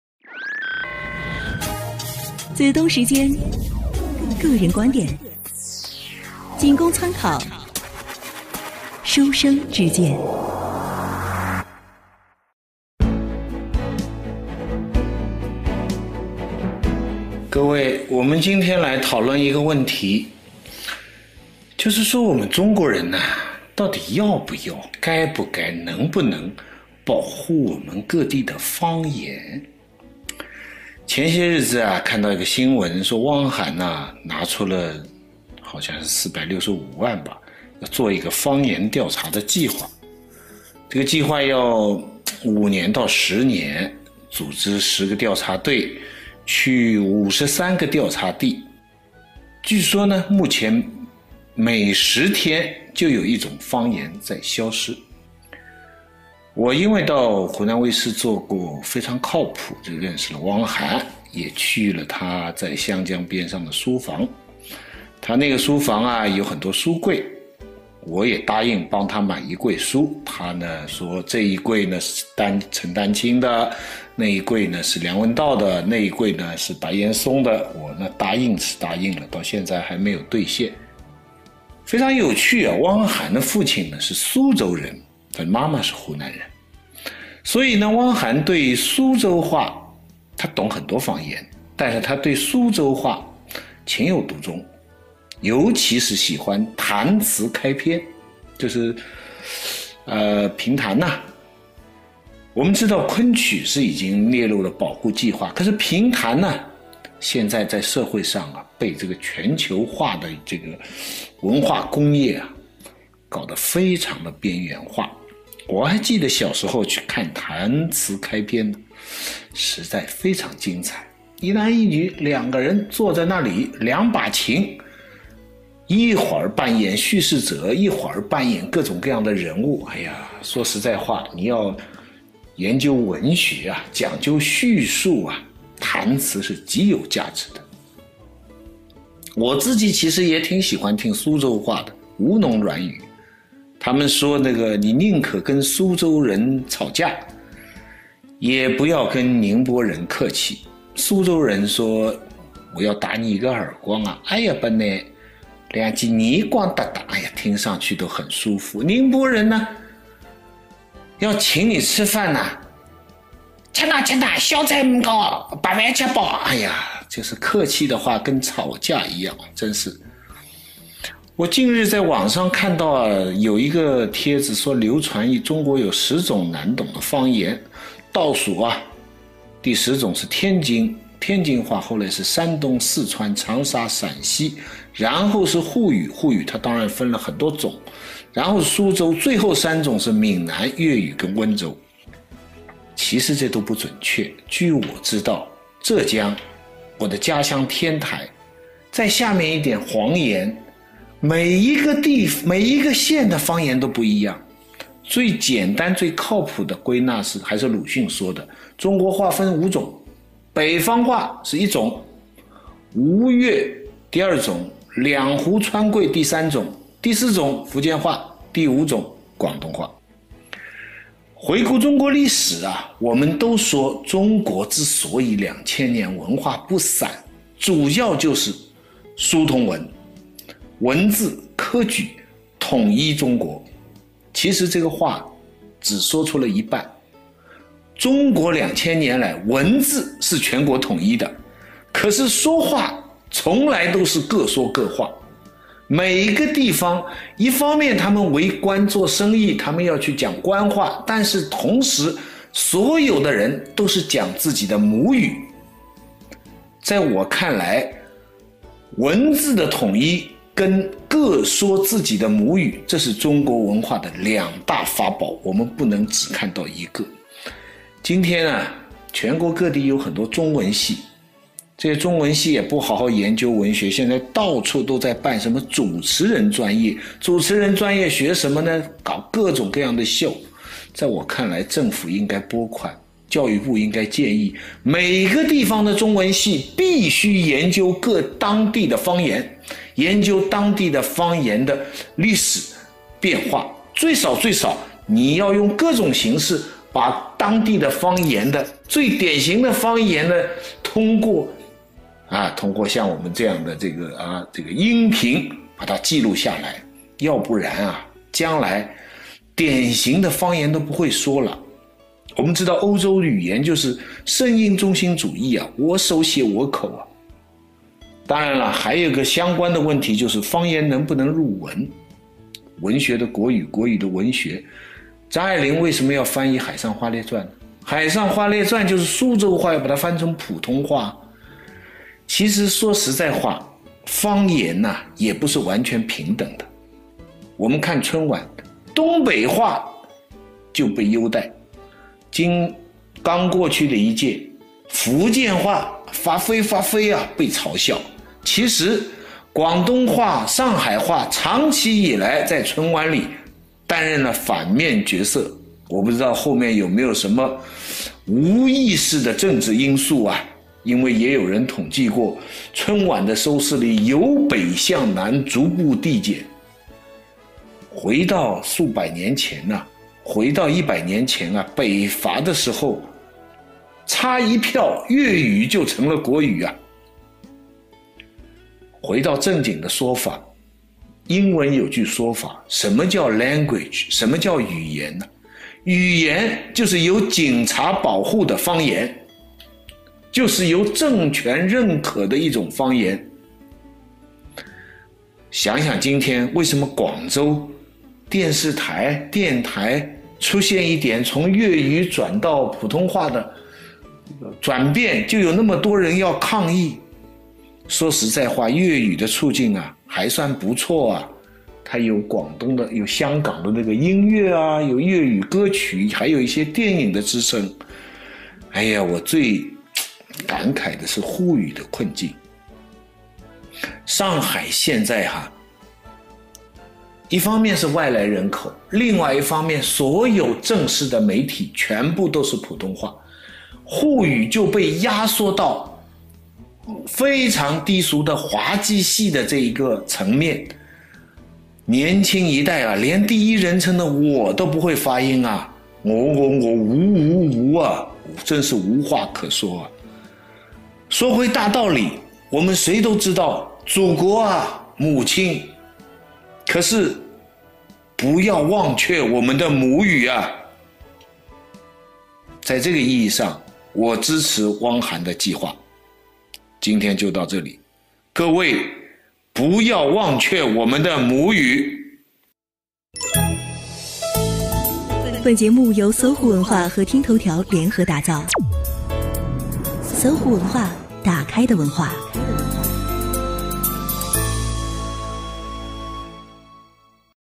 【子东时间】第5期：为何东北话被捧闽南语被笑
整理自網絡,如果侵權,請聯繫本人刪除